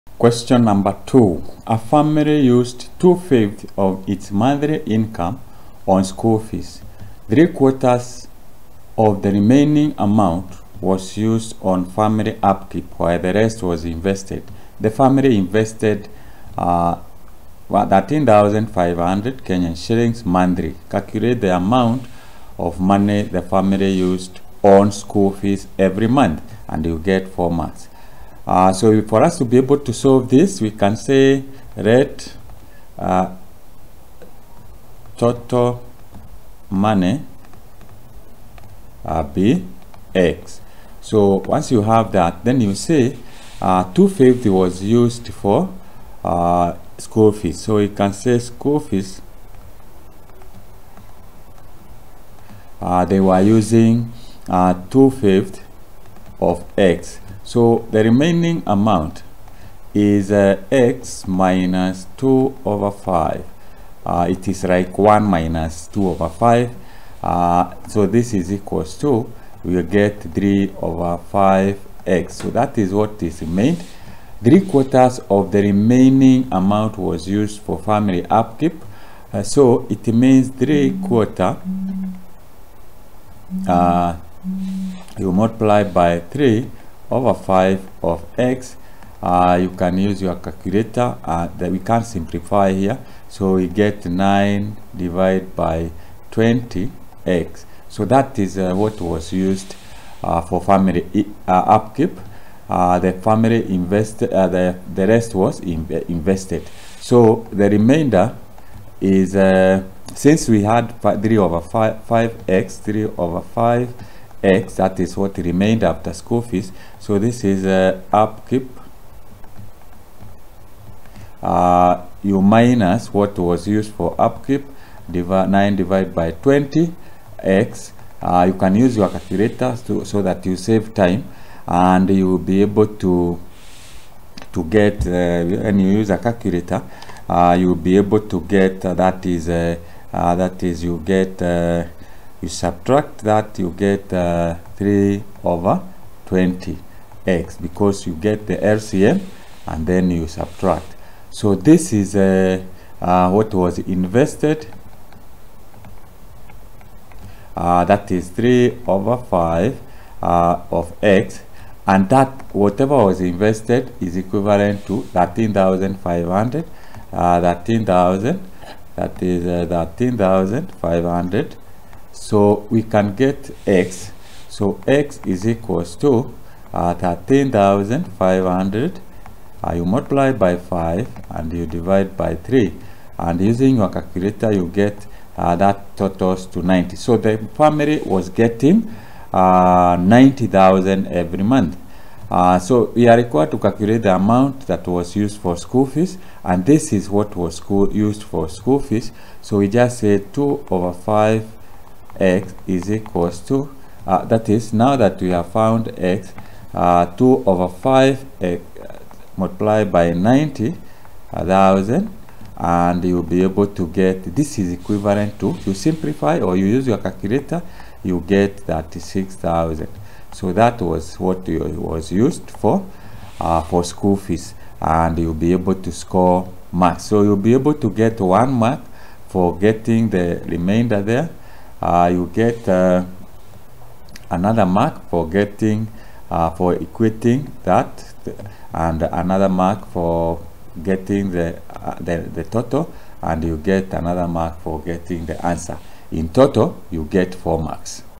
2022 KCSE MATHEMATICS PAPER 1 QUESTION 2
This video explains how to arrive at the correct answer for Question 2 in KNEC's 2022 KCSE Mathematics Paper 1 Exam.